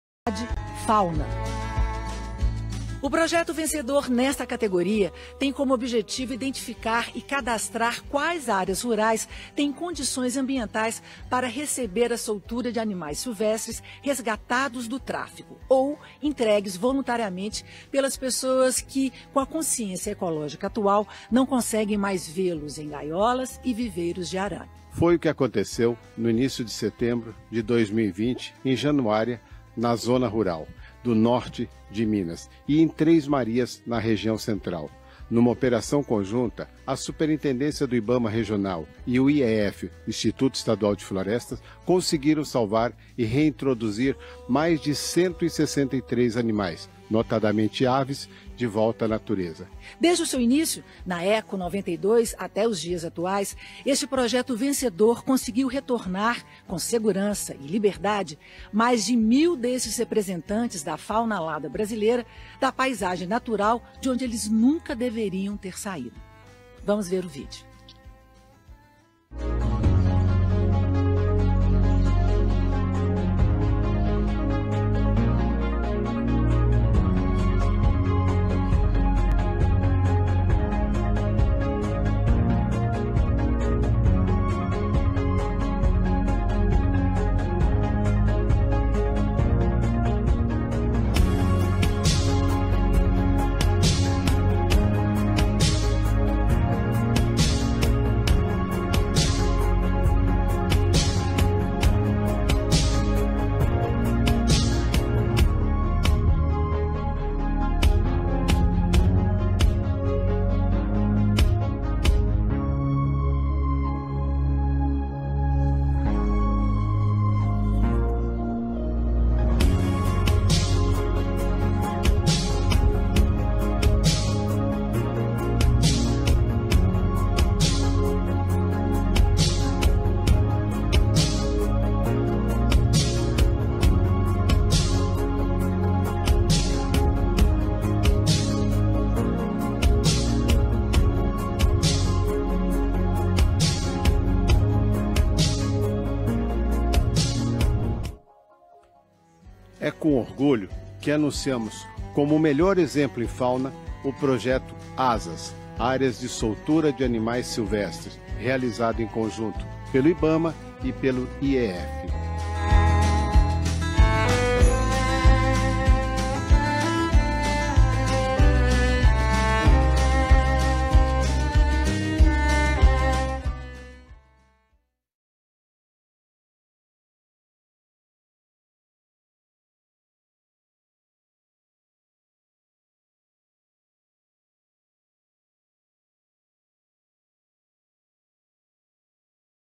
Projeto Áreas de Soltura de Animais Silvestres (ASAS) - Ganha o Prêmio Mario Werneck
Em uma cerimônia online, o Projeto ASAS - Área de Soltura de Animais Silvestres, do IBAMA (Instituto Brasileiro do Meio Ambiente e dos Recursos Naturais Renováveis) e do IEF-MG (instituto Estadual de Florestas de Minas Gerais) foi anunciado como o vencedor do prêmio Hugo Werneck na categoria 'Biodiversidade- Fauna'!!

Nós do WAITA estamos imensamente felizes e orgulhosos de poder fazer parte e apoiar um projeto tão lindo, que têm uma grande importância para a conservação da fauna mineira, acompanhados de pessoas tão especiais!

Continuemos lutando pela liberdade! Muito obrigada a todos que nos apoiam e parabéns a nossa equipe, que luta junta por mais vidas livres!